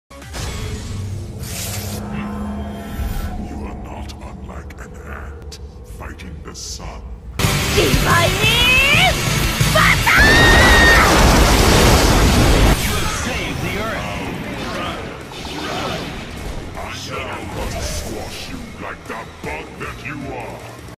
Subaru Nakajima takes Galactus down with Divine Buster
This video shares the same history with the last one.
And by that, I mean I would've made a video that involves Subaru Nakajima owning somebody. I was thinking of having her own Magneto (because, let's face it, what kind of Marvel fan don't know Magneto), but I realize the footage I've decided to use for Subaru wouldn't be a good transition to scene where the character get owned. It's kinda hard to explain so you're gonna have to watch some Ownage videos like this where a villain gets assaulted, but yeah. Because of this reason, I decided to have Subaru own nobody.
But then I remember Galactus and figured that he would be perfect for this video. So that's what I did here. Besides, I've only owned the Devourer of Worlds once prior to this vid.